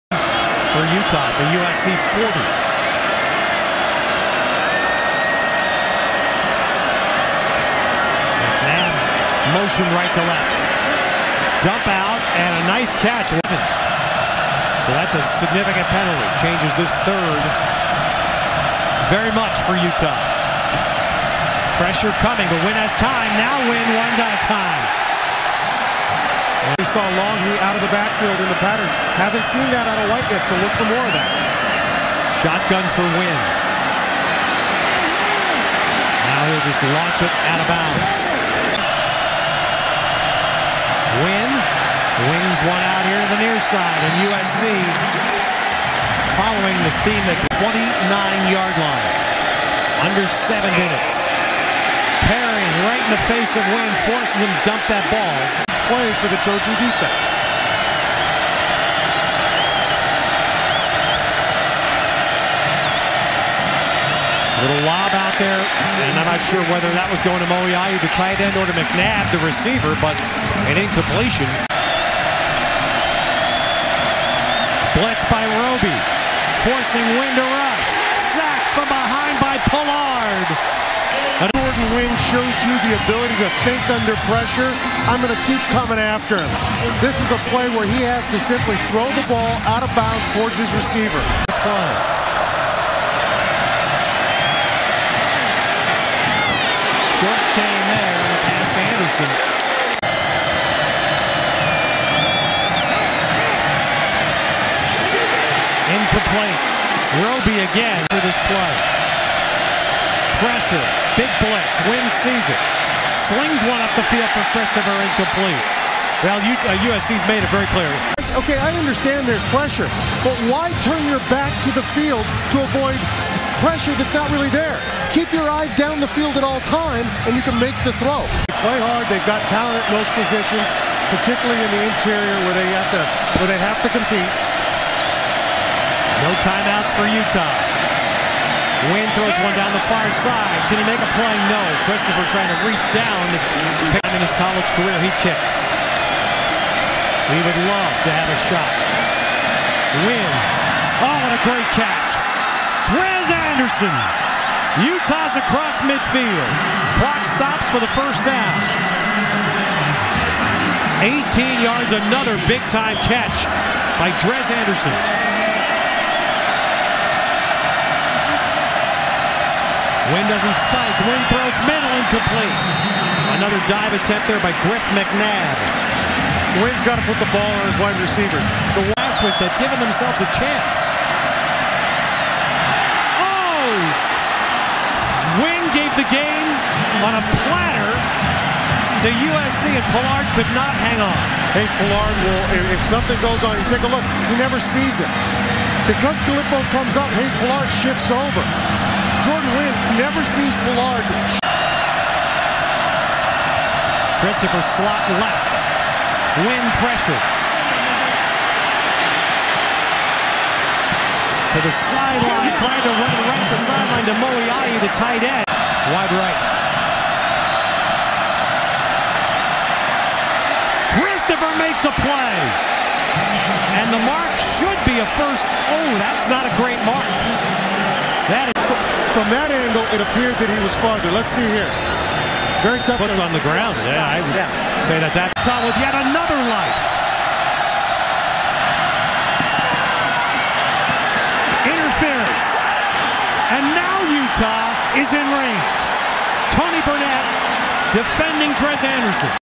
Jordan Wynn's difficult 4th quarter vs. USC
Utes had 5 different possessions in the 4th quarter and the offense came away with 0 points. Unacceptable.